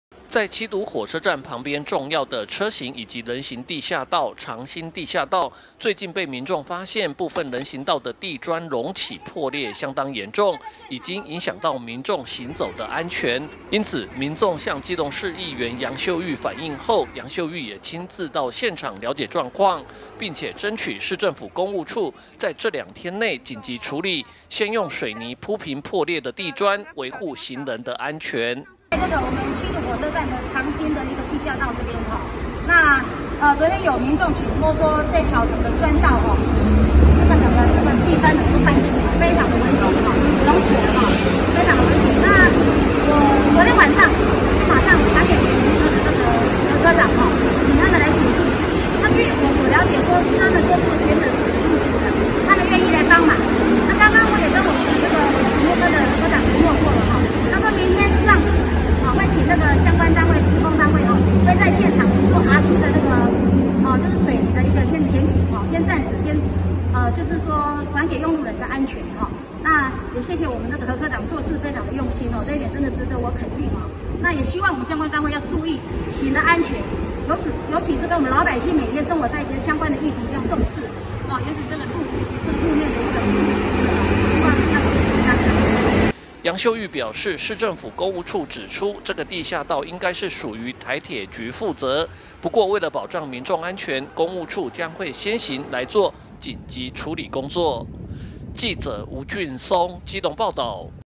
基隆交通|七堵交通|地下道安全|人行道地磚破裂|七堵火車站|長興地下道人行道破裂 楊秀玉爭取緊急處理
長興地下道人行道破裂 楊秀玉爭取緊急處理

(中嘉吉隆新聞記者吳俊松/基隆報導)在七堵火車站旁邊，重要的車行以及人行地下道-”長興地下道”，最近被民眾發現，部分人行道的地磚隆起破裂相當嚴重，已經影響到民眾行走的安全，因此楊秀玉也親自到現場了解狀況，並且爭取市政府工務處在這兩天內緊急處理，先用水泥鋪平，維護行人的安全！